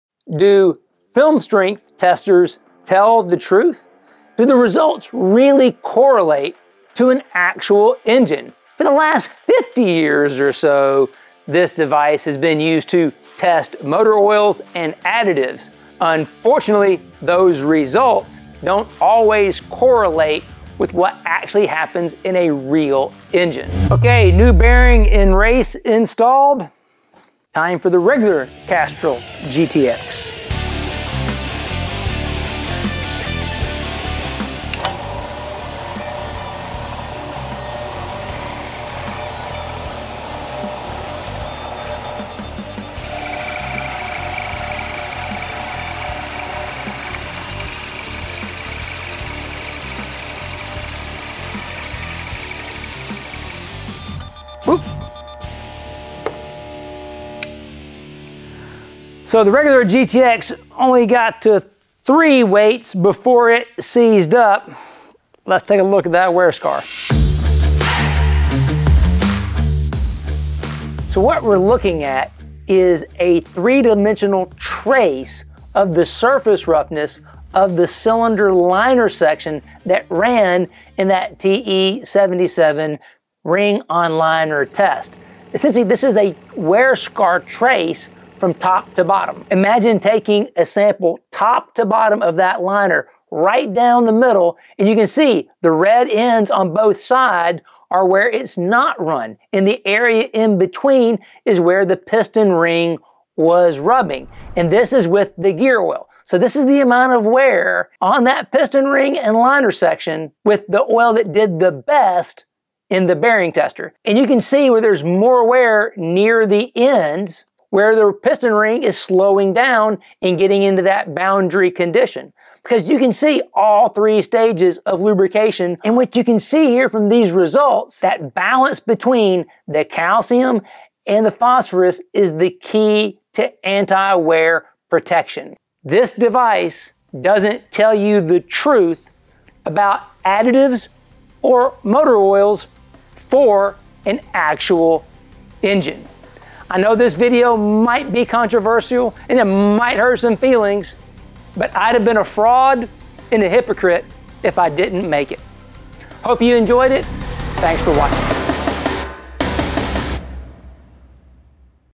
Do “Film Strength” Testers Tell The TRUTH?
Do “Film Strength” test results correlate to an actual engine? This device was developed almost 100 years ago to evaluate gear oils and greases. Today, many people and products use “film strength” data to make product claims and evaluations, let's see what the science says about this device for motor oils.